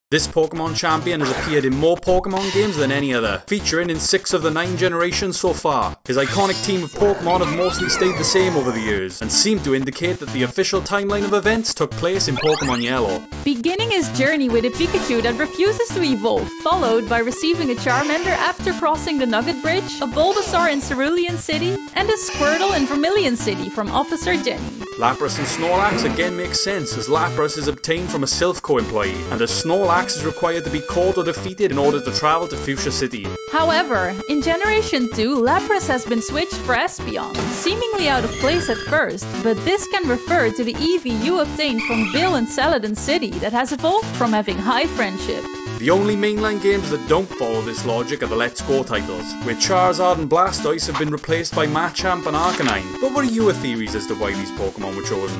This Champion Lost 2 Of His Iconic Pokémon
Champion Red has appeared in more Pokémon games than any other, and his team of Pokémon has mostly stayed the same over the years. However, in the latest games his lineup has had some surprising replacements. What are your theories as to why these Pokémon were chosen?